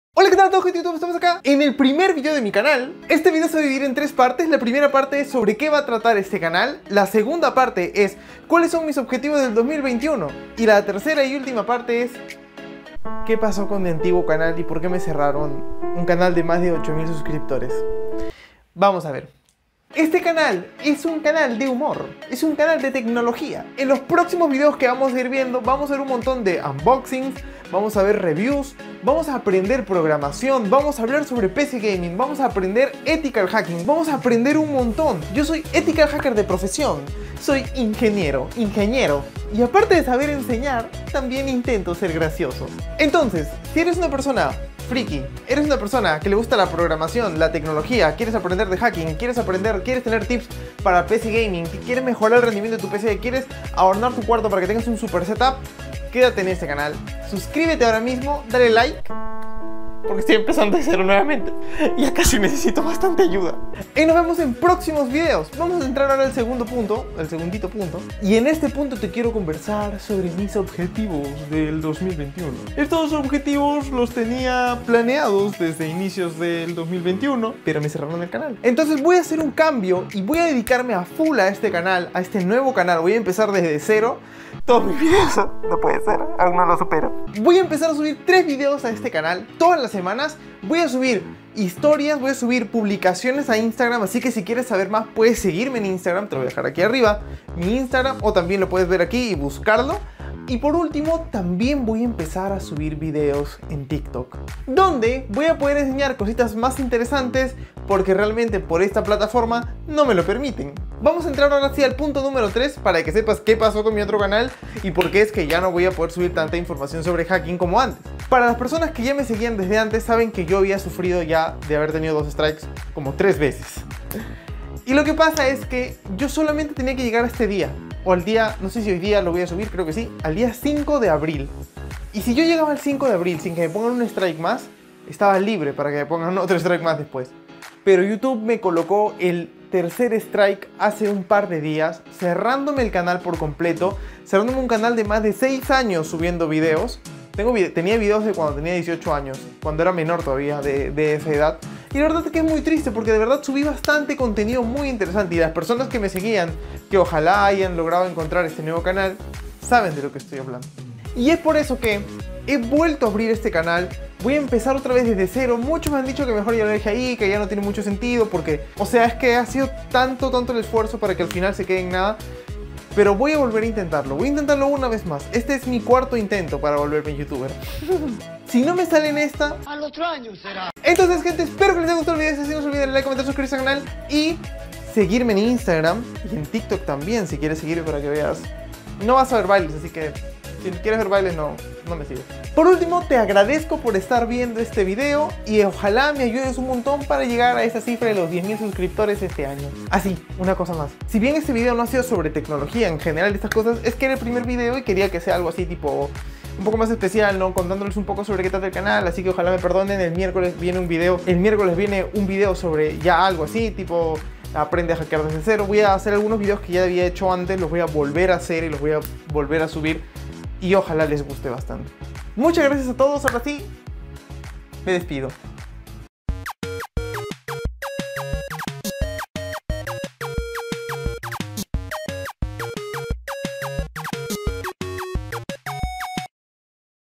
El regreso de Anormalix | Mi nuevo Canal
⏰ MARCAS DE TIEMPO:
0:00 Inicio
0:19 Sobre que es el canal?
1:06 Objetivos del canal
1:51 Que paso con mi otro canal?